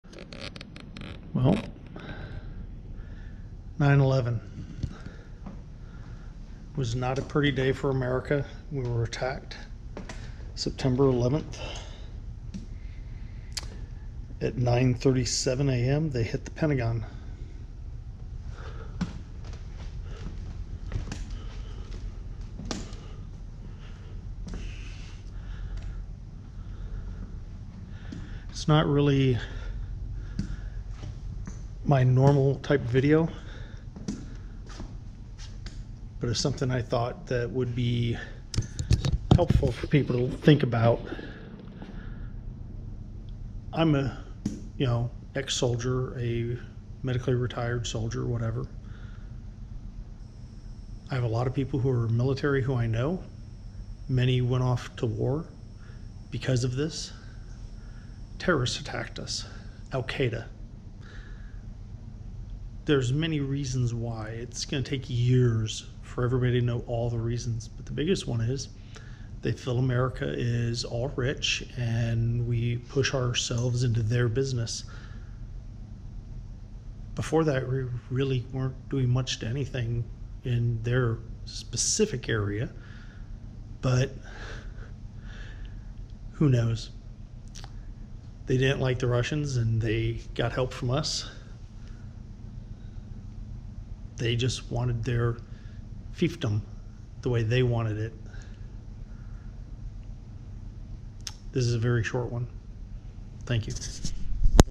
9/11 memorial thinking
Memories from 9/11 for all to think about.

Memorial Photos from the Navy Museum memorial area.